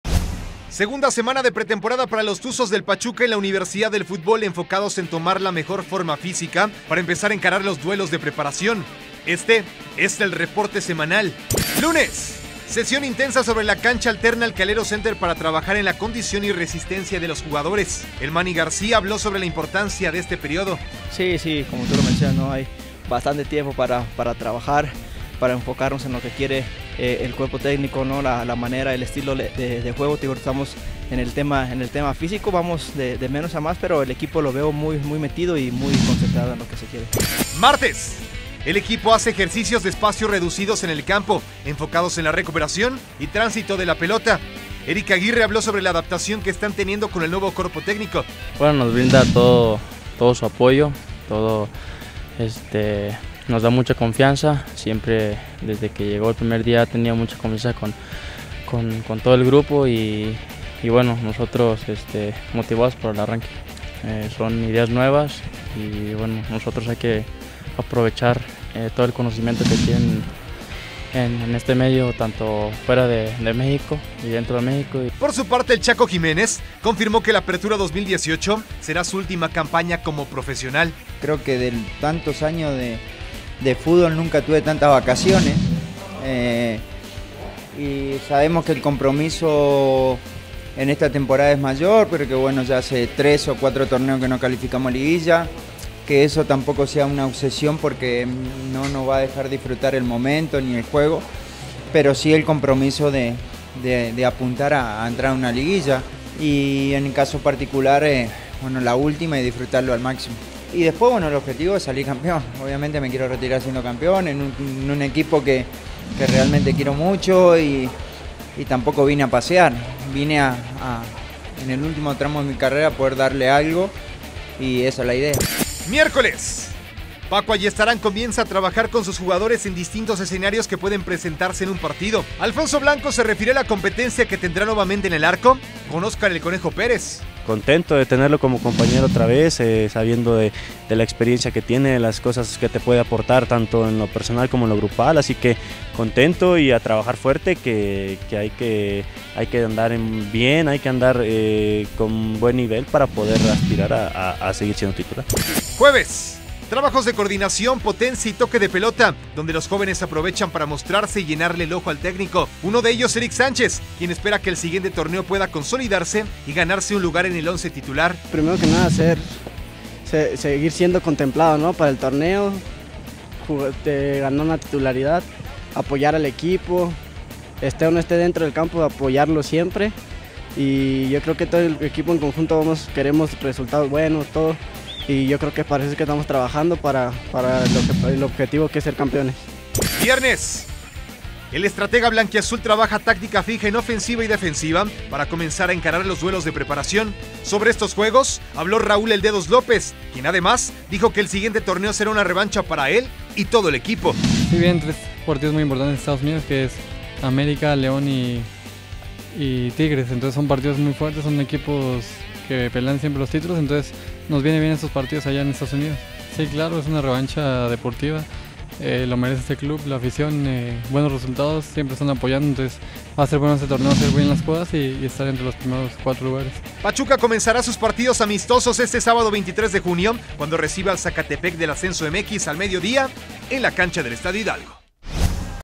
Tuzostv: Reporte Semanal
Checa cómo fue la preparación de los Tuzos en su 2da semana de Pretemporada previo a su primer encuentro amistoso.